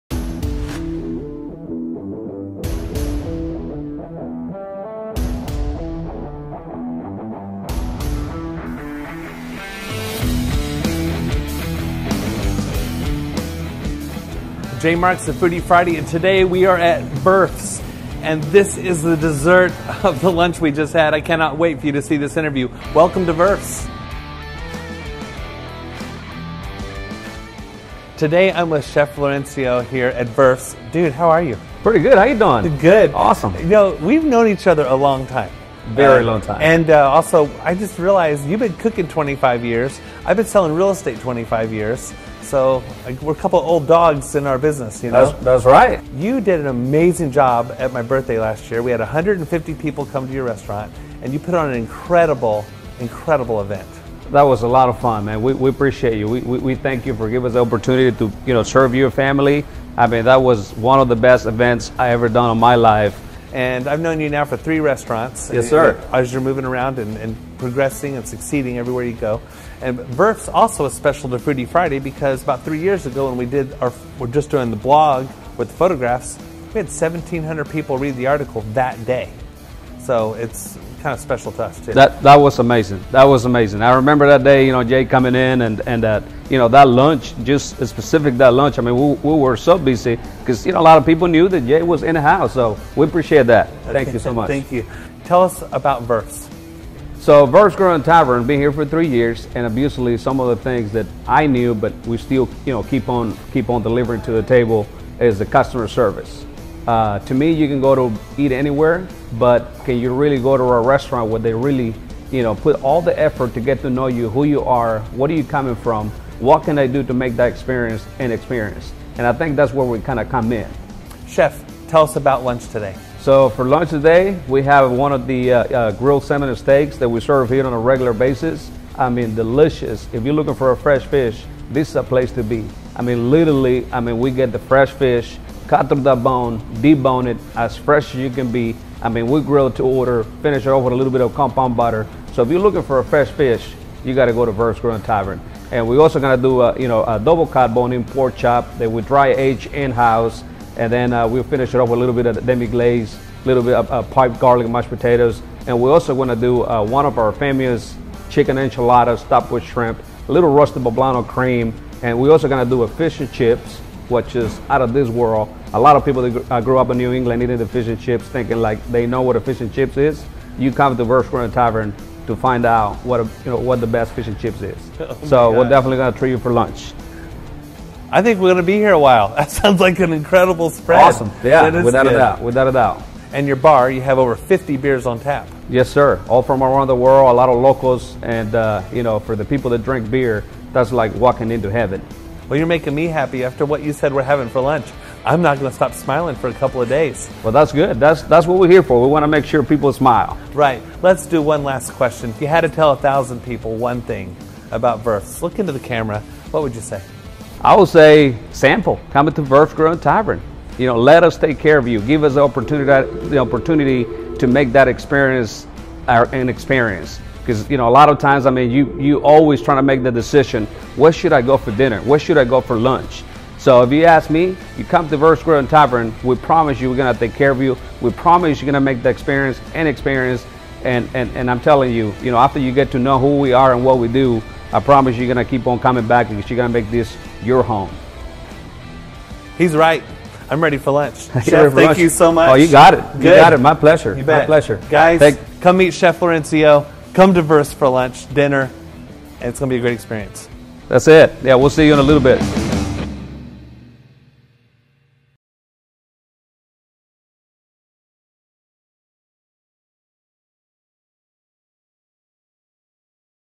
Foodie Friday DFW: Verf's Grill & Tavern
Jay Marks with Foodie Friday DFW visits with Chef Florencio at Verf's Grill & Tavern in Flower Mound, TX.

This tavern-style restaurant has everything you need for date night, a business lunch, or just a fun night out with friends and/or family.